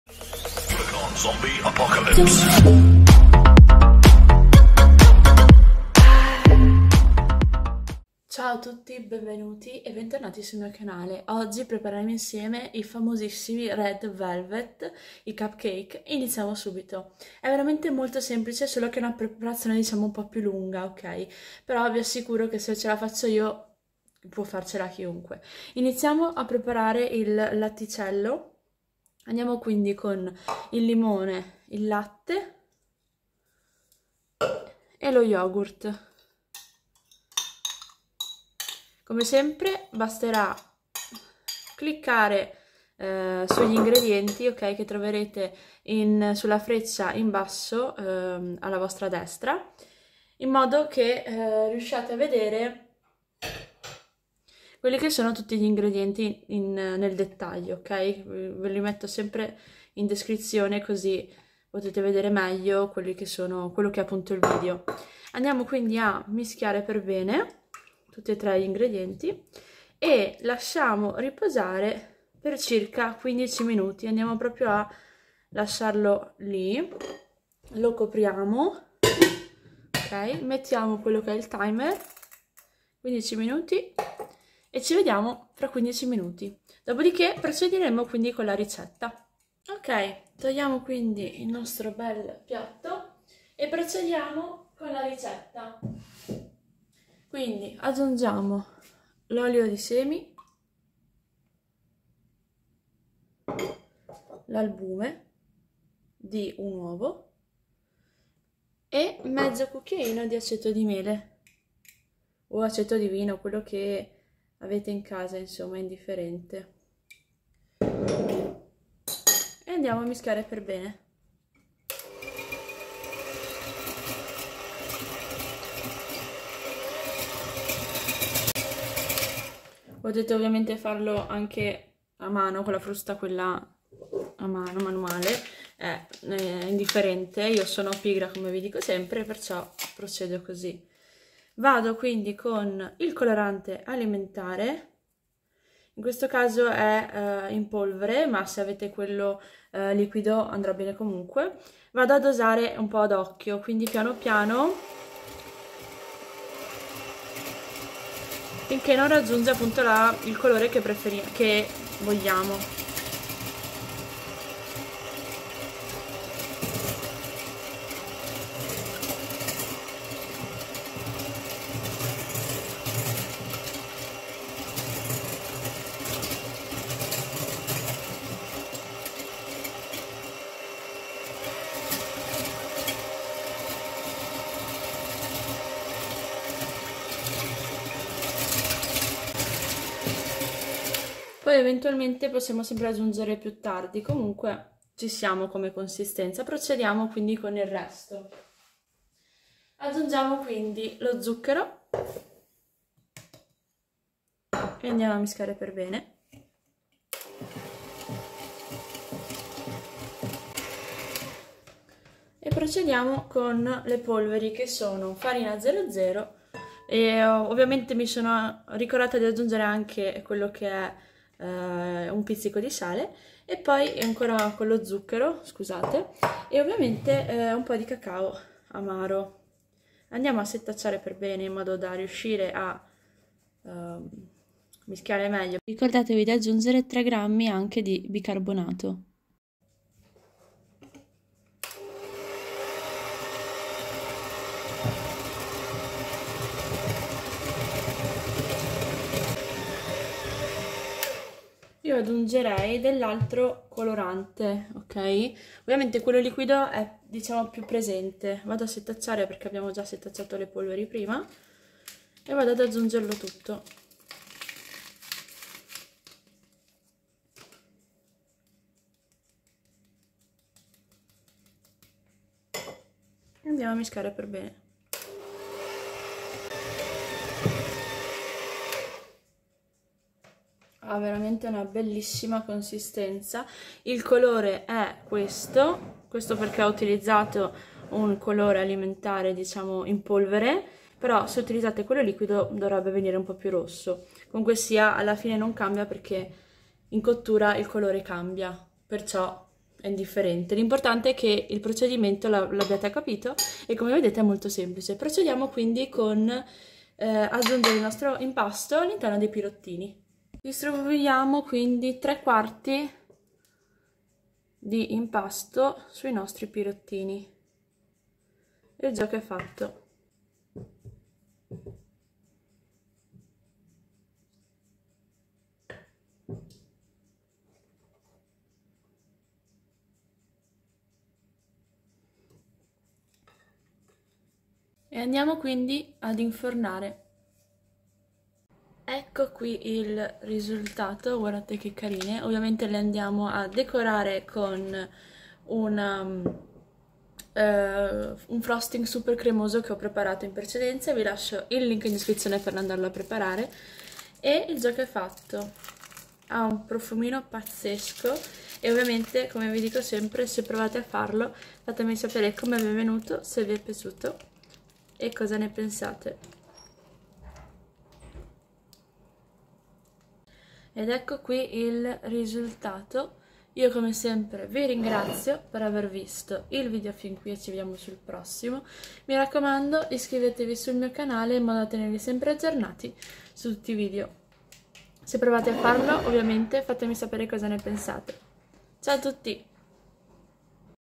RED VELVET CUPCAKE -FACILI, MORBIDI E BUONI-
Ingredienti:
60g di yogurt bianco
60g di latte
10g di succo di limone
110g di olio di semi
1 albume
1/2 cucchiaino di aceto di vino
3g di bicarbonato
(puoi sostituire l'aceto e il bicarbonato con 1 cucchiaio di lievito per dolci)
colorante rosso (in gel meglio)
135g di zucchero
155g di farina 00
5g di cacao amaro

Ring light
Scheda audio